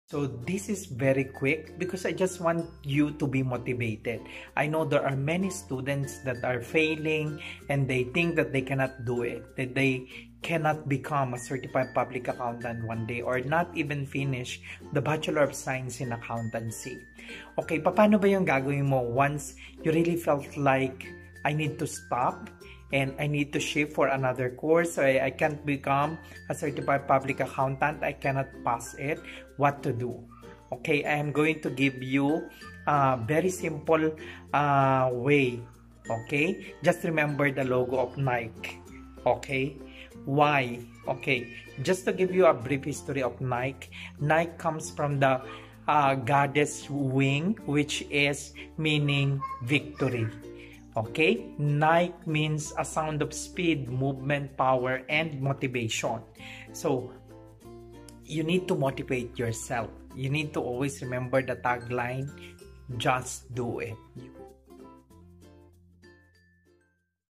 Just Do It for CPA Dreams!
Accounting Coach Survival Tips, Jonathan Ruiz, CPA shared motivational and inspirational words.

Don't lose motivation.